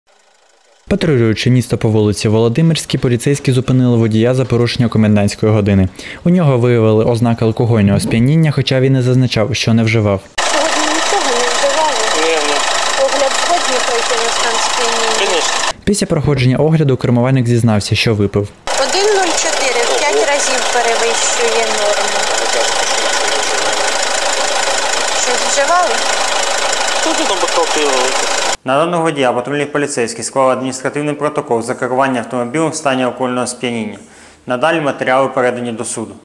У Сумах нетверезий водій порушив режим комендантської години
У Сумах по вулиці Володимирській поліцейські зупинили водія за порушення комендантської години. У нього виявили ознаки алкогольного сп’яніння. Огляд зафіксував позитивний результат – 1,04 проміле, що у 5 разів перевищує норму. На водія інспектори склали адмінпротокол.